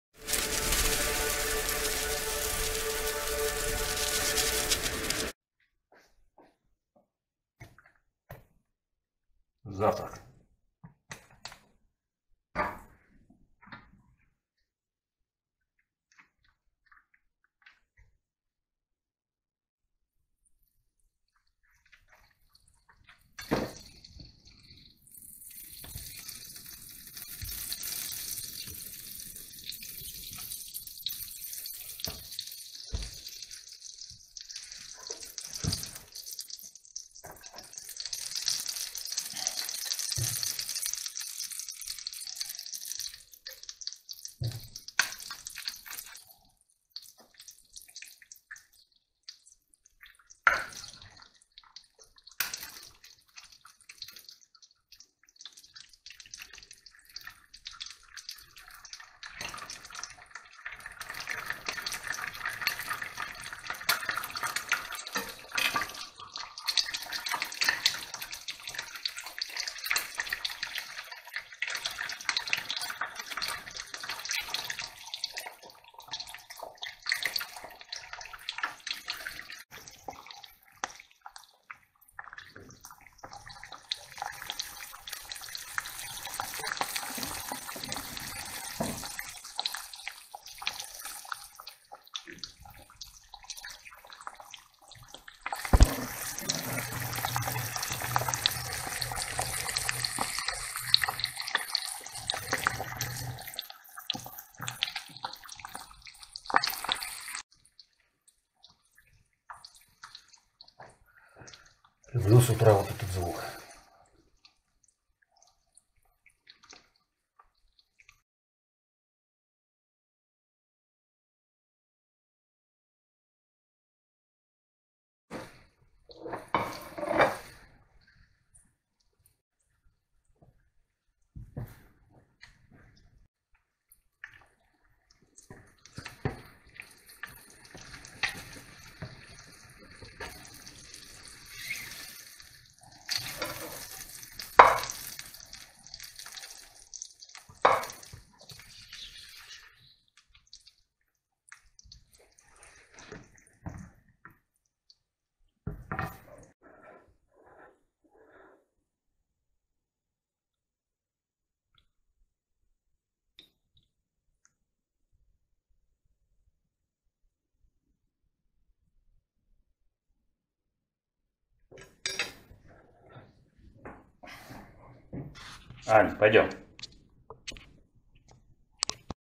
ЯИЧНИЦА ГЛАЗУНЬЯ ВЫПУСКНАЯ 🥚🥚🥚
Яи́чница — блюдо, приготовляемое на сковороде из разбитых яиц.
Яичница, в которой желток не смешан, не сболтан с белком.
Рецепт взят из книги ,,Ручная книга Русской опытной хозяйки,, К. Авдеевой 1846 г.

Яйца
Соль